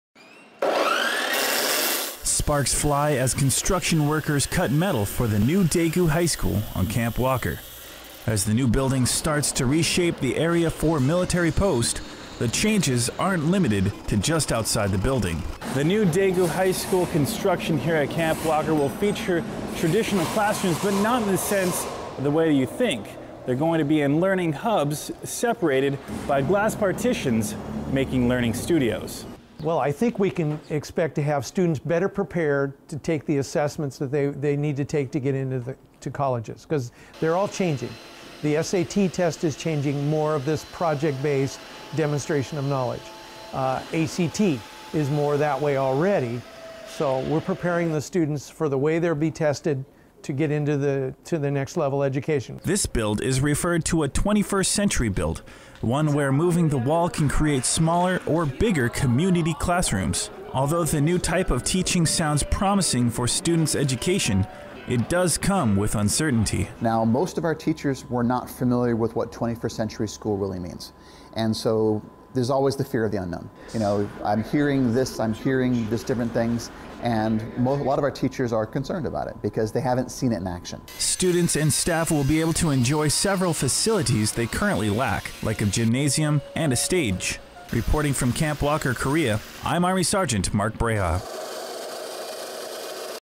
AFN Daegu - New DHS
There are many tools to help people of all ages learn. Sergeant Mark Brejcha heads to Camp Walker where construction is leading to the newest set of educational tools.